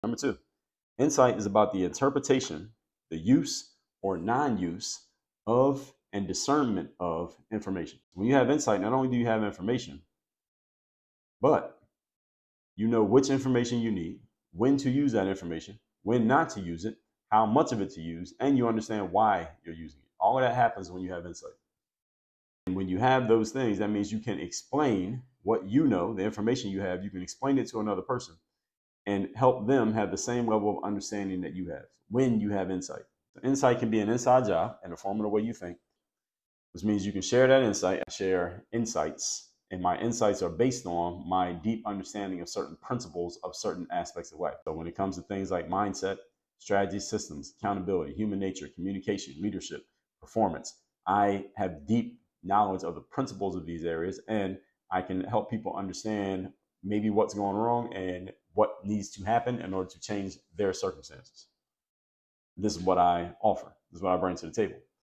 What Does Insight 💡 Really Mean? How to Unlock the Power of Information 🧠
Gain a deeper understanding of insight and its significance in decision-making. Discover how to leverage information effectively and explain complex concepts to others. Explore the principles behind mindset, strategy, communication, and more for personal and professional growth.